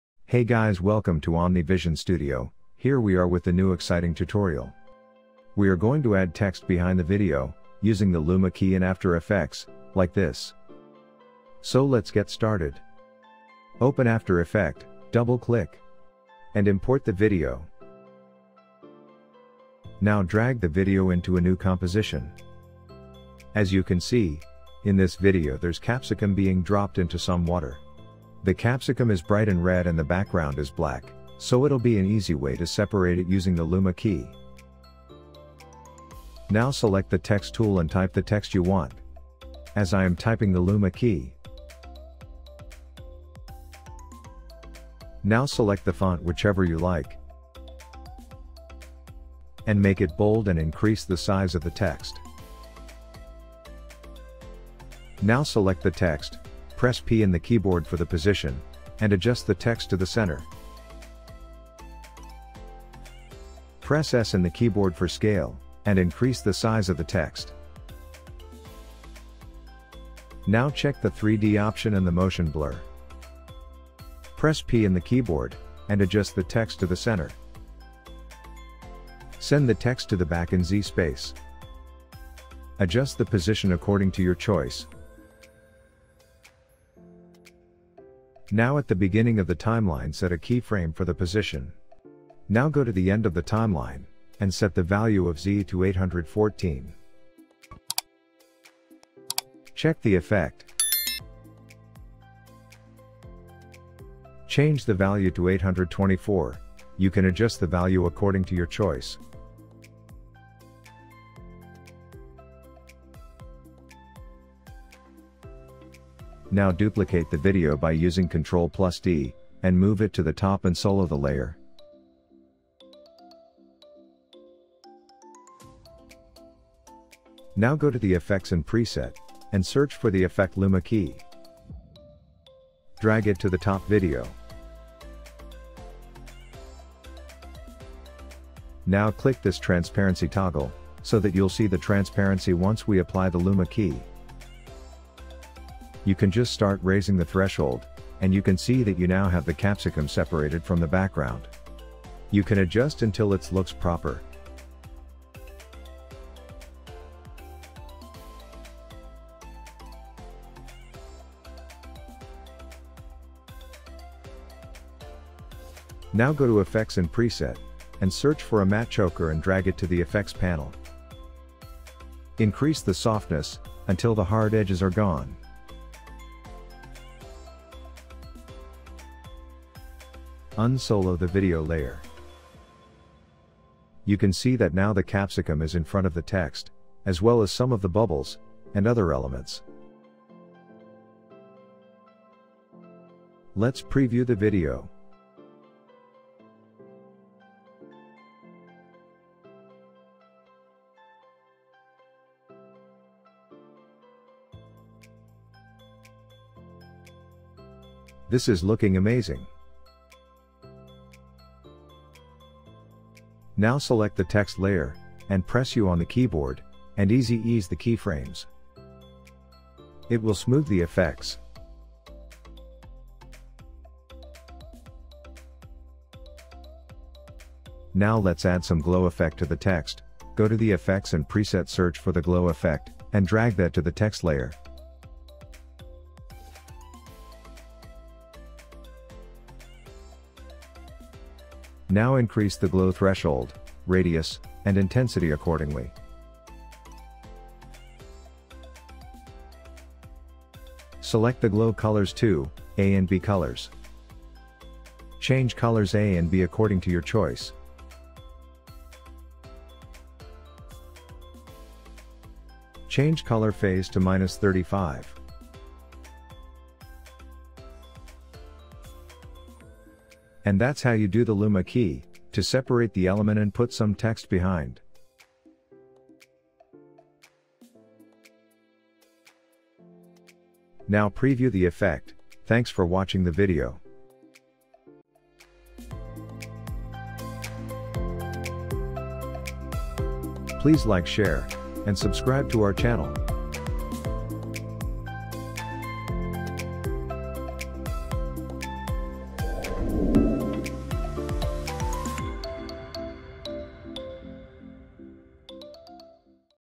How To Add Text Behind Object | After Effects Tutorial | Luma Key Effect | Removing Black Background
In this tutorial, we are going to add text behind the video.
Using After effects LUMA KEY effect.
It can be used to remove pure black background or white to mask out the object.
This technique is useful if used correctly.